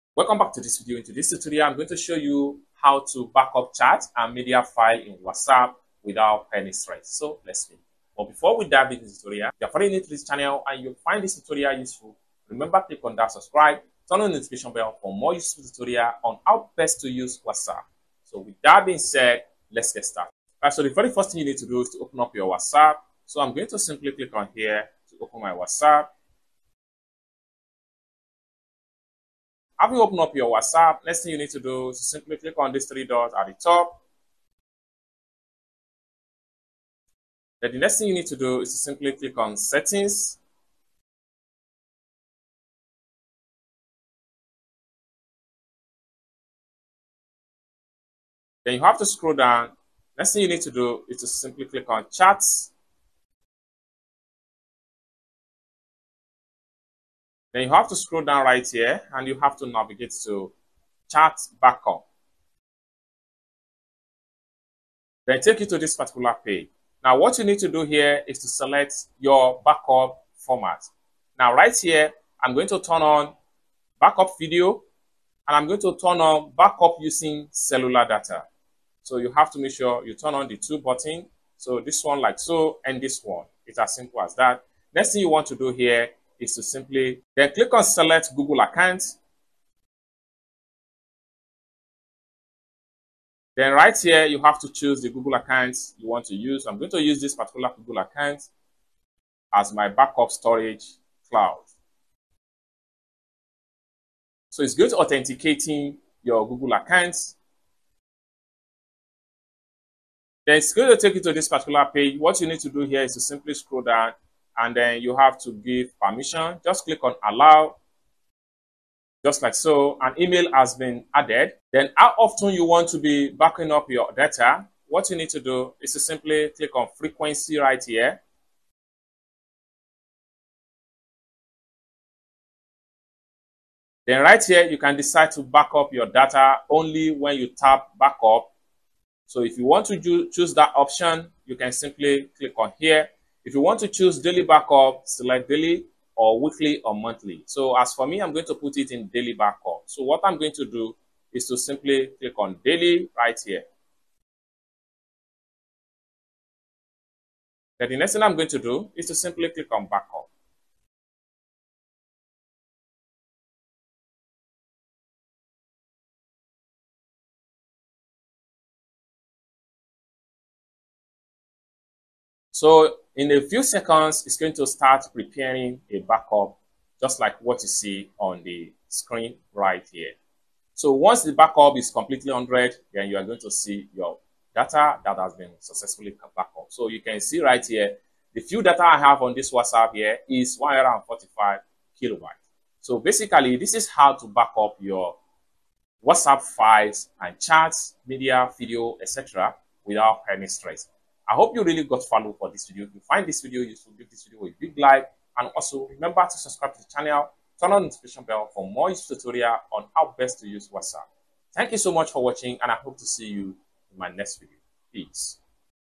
How to Back up WhatsApp Chat - Easy Tutorial
In this video, we'll guide you through the simple steps to back up your WhatsApp chats on both Android and iOS devices. Whether you're switching phones or just want to ensure your messages are safe, this tutorial has got you covered!

🔹 Why Back Up Your WhatsApp Chats?
Backing up your WhatsApp chats is crucial to prevent data loss. It ensures that all your important messages, photos, and videos are safely stored and can be easily restored if you switch devices or reinstall the app.

Drop them in the comments section below, and we'll be happy to help!